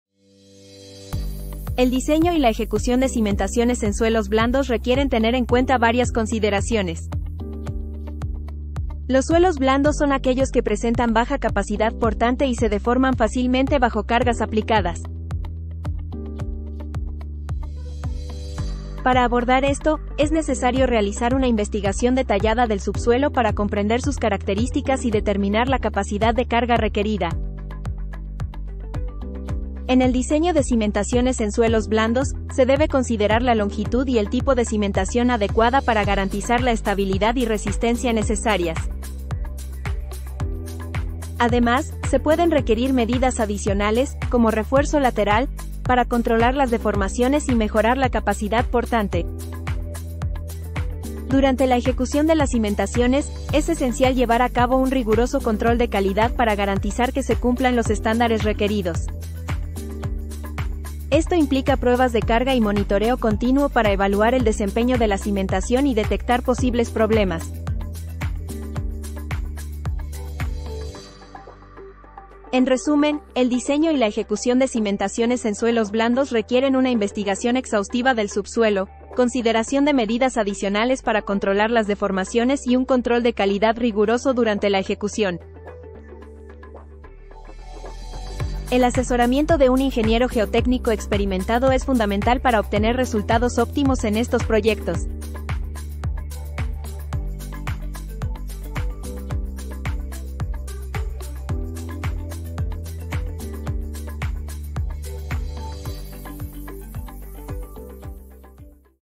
Resumen de Pilotes en Suelos Blandos
Cuando se trata de cimentaciones en suelos blandos, hay varias consideraciones importantes que deben tenerse en cuenta. Los suelos blandos, también conocidos como suelos de baja capacidad portante, son aquellos que presentan una baja resistencia al corte y se deforman fácilmente bajo cargas aplicadas.

Aquí hay algunas consideraciones clave:

 - Investigación del subsuelo: Es fundamental realizar un estudio geotécnico exhaustivo del subsuelo para comprender las características y propiedades del suelo blando. Esto incluye la determinación de la capacidad portante, la compresibilidad y la consolidación del suelo.

- Distribución de cargas: Debido a la baja capacidad portante de los suelos blandos, es importante distribuir las cargas de manera uniforme para evitar asentamientos diferenciales y fallas en la estructura. Esto se puede lograr utilizando una cimentación más amplia o mediante el uso de técnicas de redistribución de cargas, como los losa de compensación.

- Mejoramiento del suelo: En algunos casos, puede ser necesario mejorar las propiedades del suelo blando para aumentar su capacidad portante. Esto se puede lograr mediante técnicas de mejoramiento del suelo, como la compactación dinámica, la precompresión y el uso de columnas de grava o pilotes de fricción.

- Consideraciones de drenaje: Los suelos blandos tienden a tener una alta capacidad de retención de agua, lo que puede provocar problemas de consolidación y asentamiento. Es importante proporcionar un buen drenaje para reducir la presión de agua en el suelo y minimizar los efectos negativos.

- Monitoreo: Dado que los suelos blandos son propensos a deformaciones y asentamientos, es importante realizar un monitoreo continuo de la cimentación y la estructura para detectar cualquier movimiento o cambio en las condiciones del suelo.

Es importante tener en cuenta que estas consideraciones son generales y pueden variar según las características específicas del sitio y el proyecto.